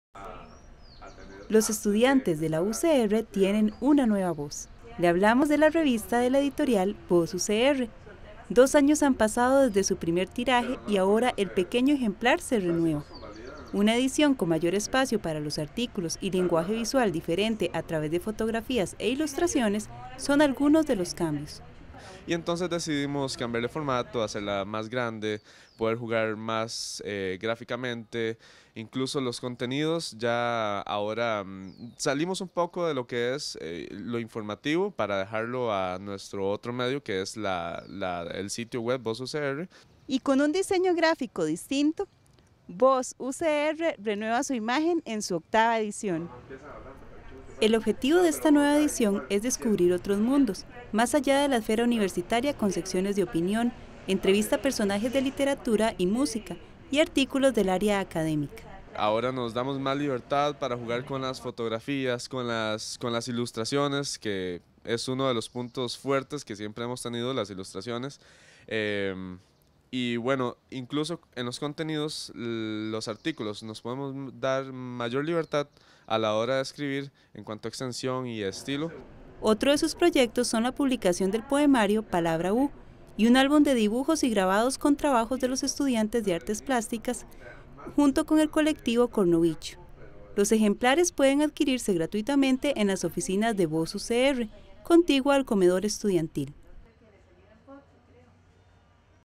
Revista Voz UCR se renueva
Un nuevo lenguaje visual es uno de los cambios que usted verá en las páginas de la Revista Voz UCR.